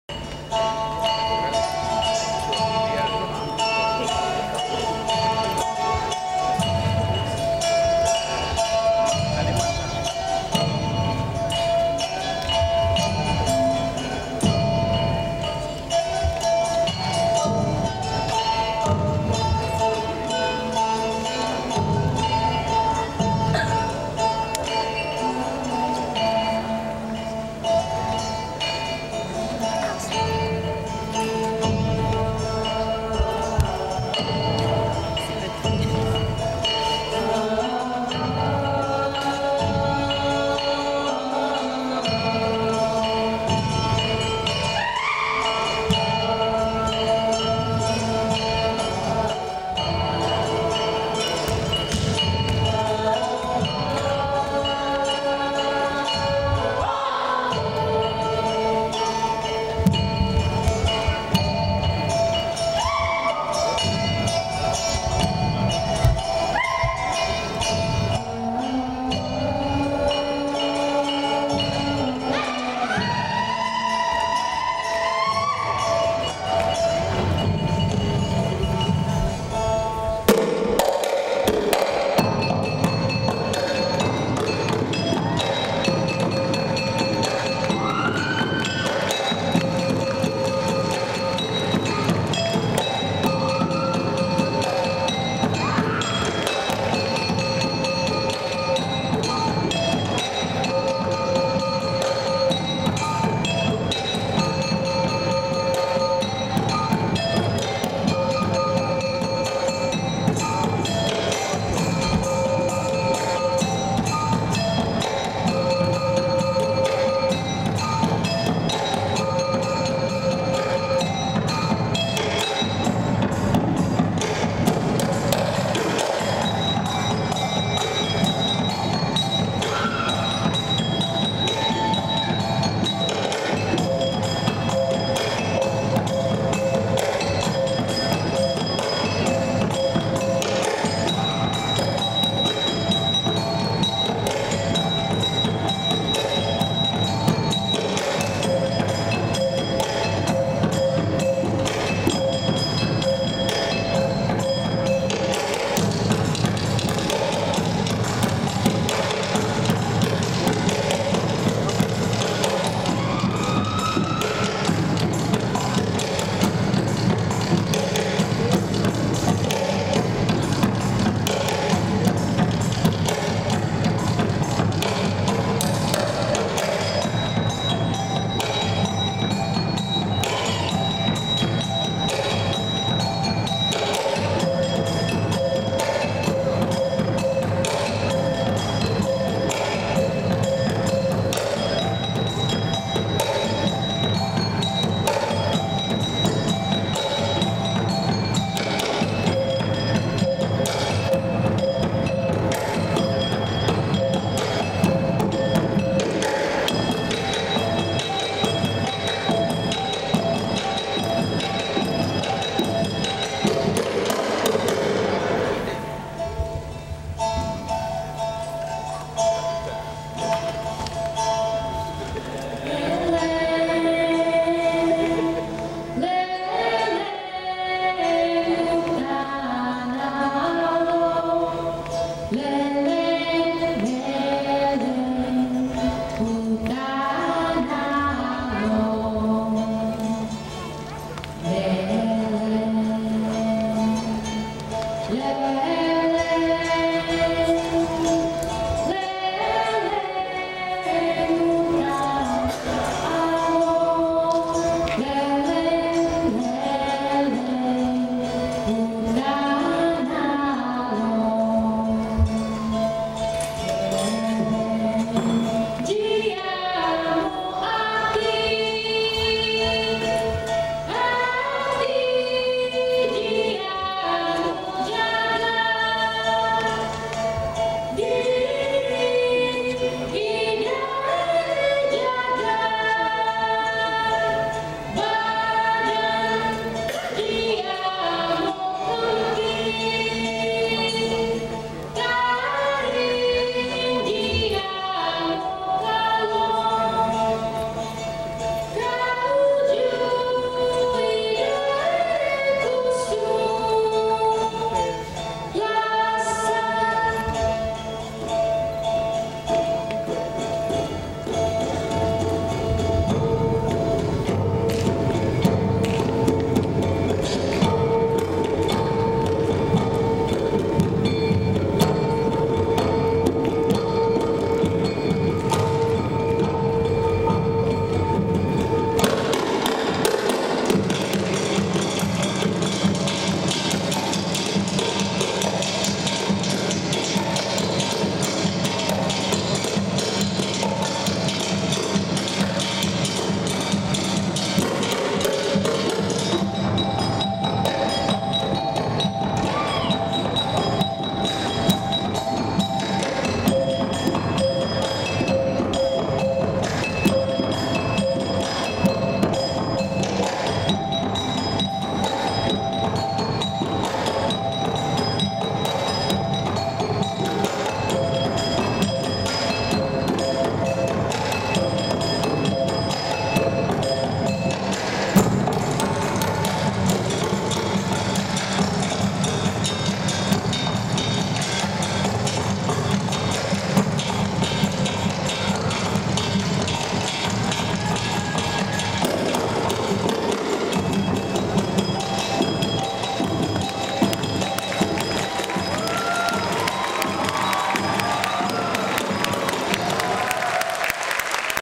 tari enggang.VOB
Tari Enggang
feathers dance, hornbills dance
Saung Budaya Indonesian Dance Group
Dirgahayu Indonesia ke 66
York College Performing Arts Center
New York
25 September 2011